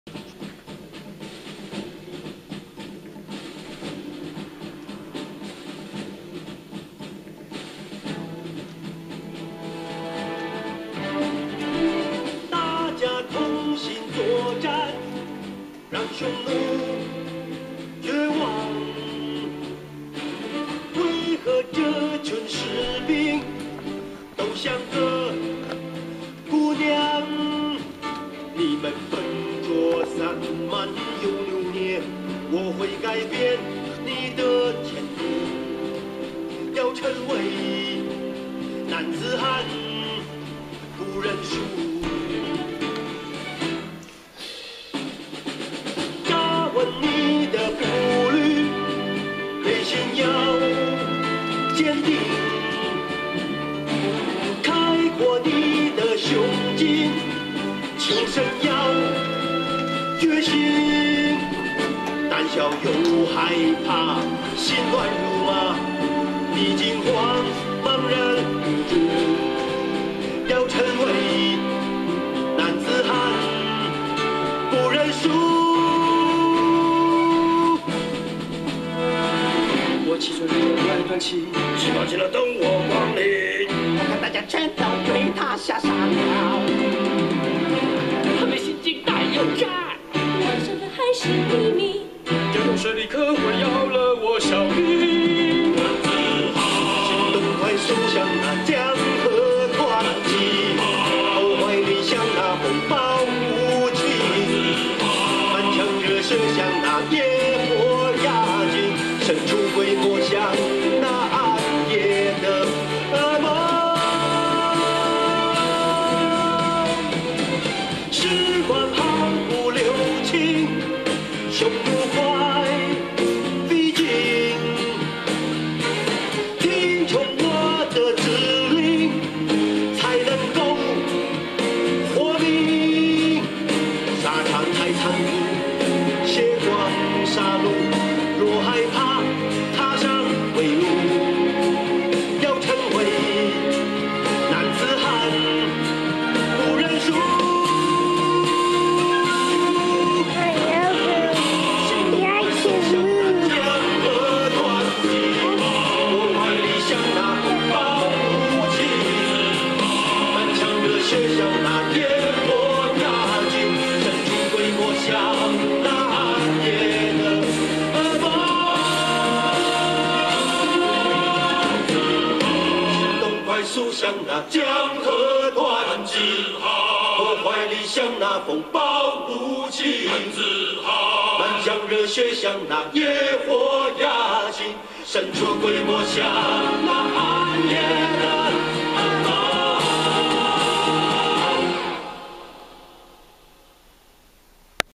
Jackie Chan Kung Fu song
No bad words! Just great kung fu moves from Jackie Chan